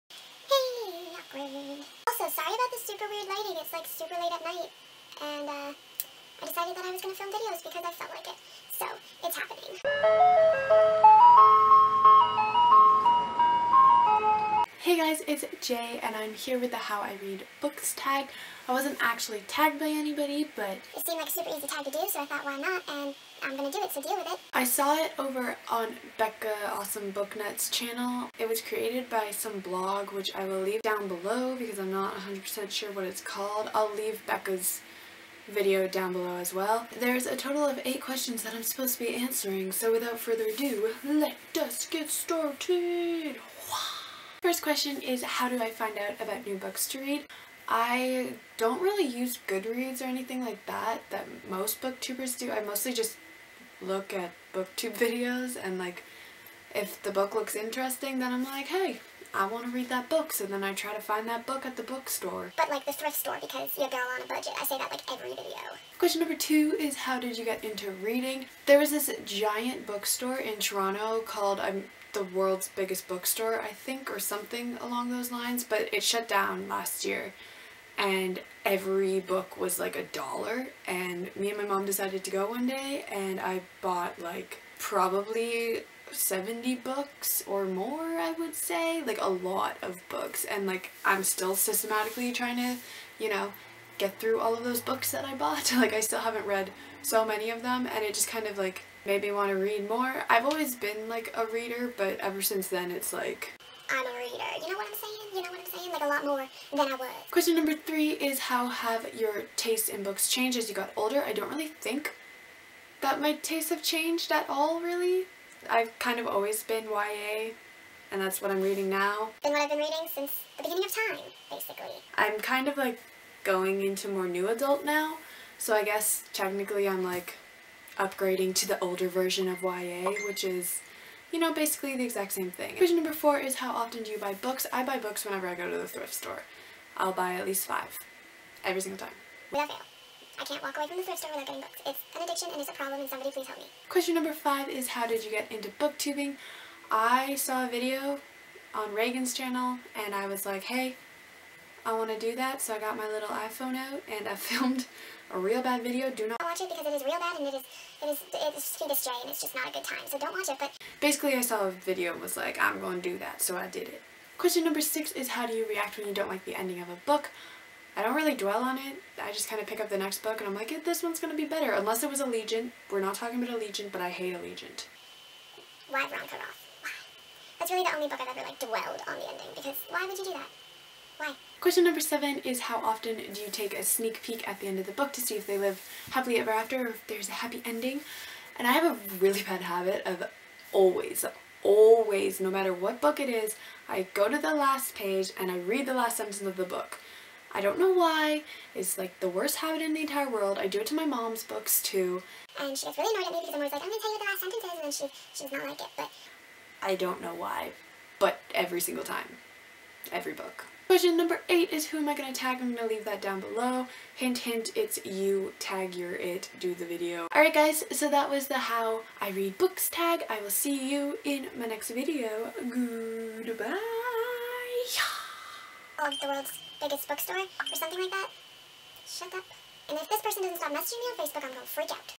THE HOW I READ BOOK TAG | FETUS JAY FRIDAY
LOOK HOW SMOL!

I can't find the original video... I am so sorry

Questions:
1. How do you find out about new books to read?
2. How did you get into reading?
3. How has your taste in books changed as you've gotten older?
4. How often do you buy books?
5. How did you get into Booktubing?
6. How do you react when you don't like the end of a book?
7. How often do you take a "sneak peak" at the end of a book to see if there is a happy ending?
8. Who are you going to tag?